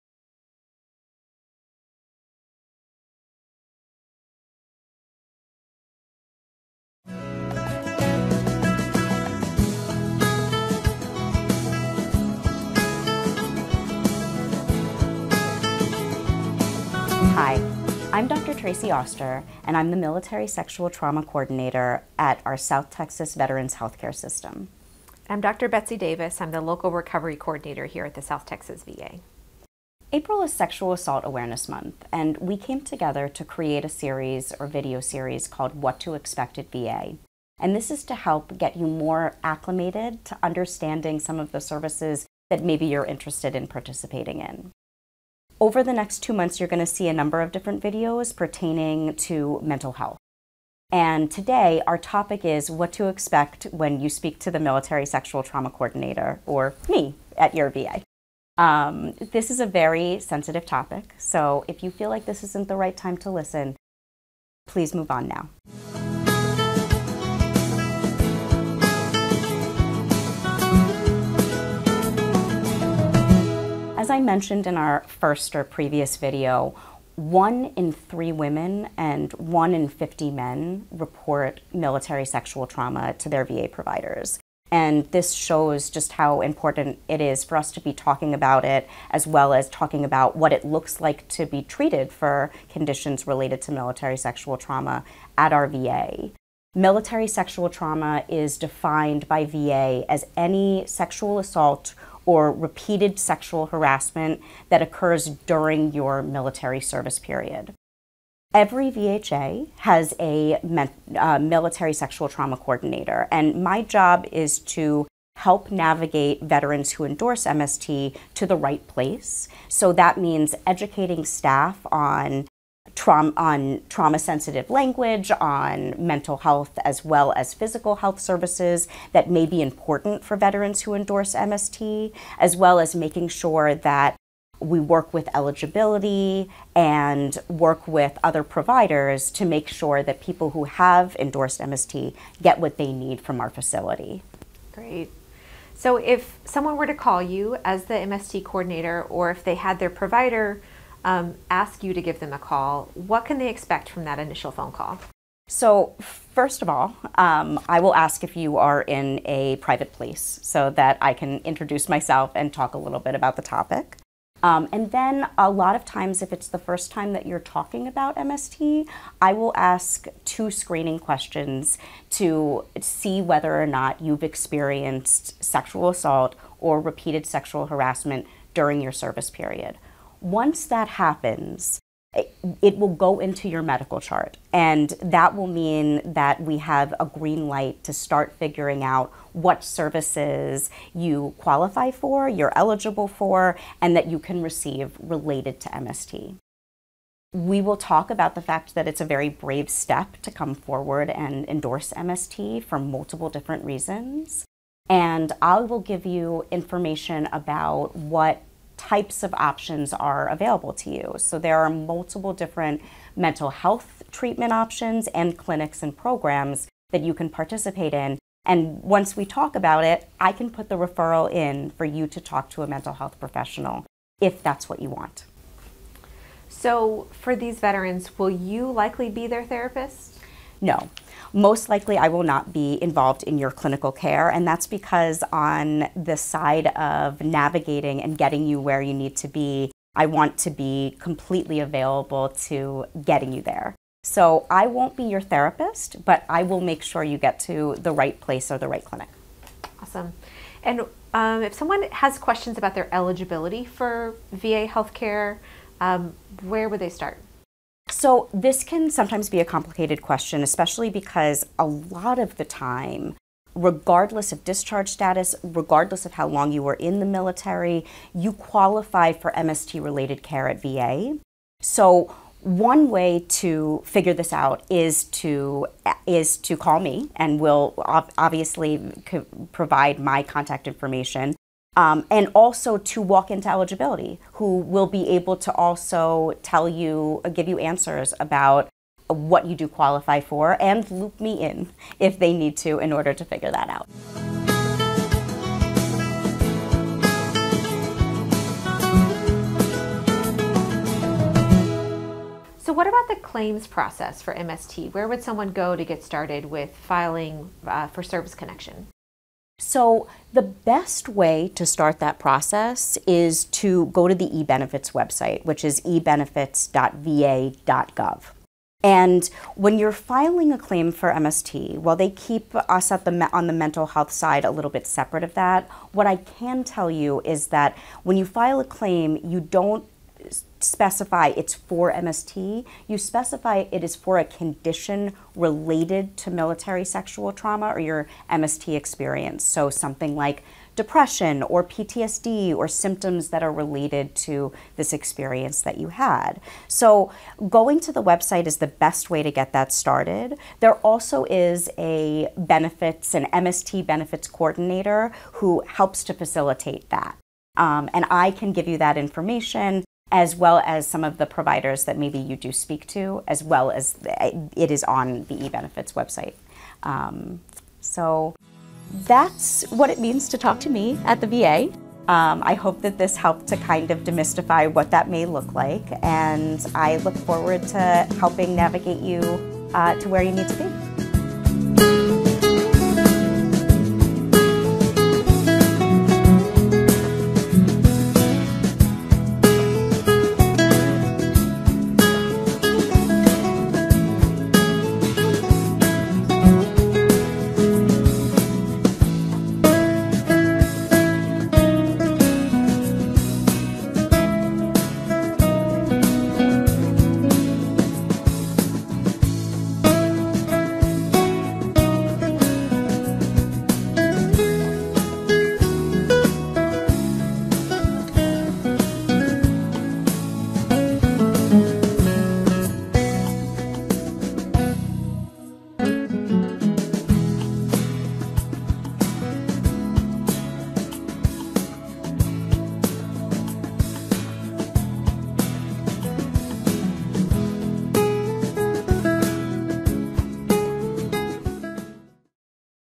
What to expect when speaking with an MST Coordinator
April is Sexual Assault Awareness Month and and this video will help demystify any of the services a person may be interested in the VA when a person needs to talk to a Military Sexual Trauma (MST) Coordinator.